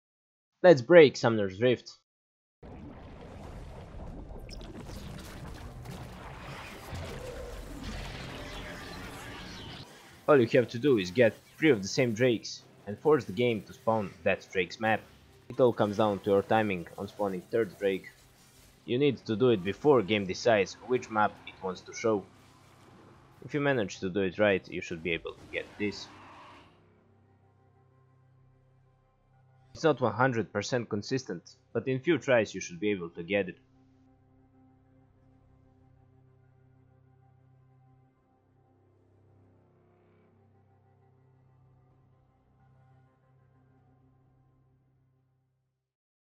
Breaking the map in 2024.
It's possible to delete floor part of the map in practice tool, this bug with dragons confuses the game, and it doesn't load any model for the map.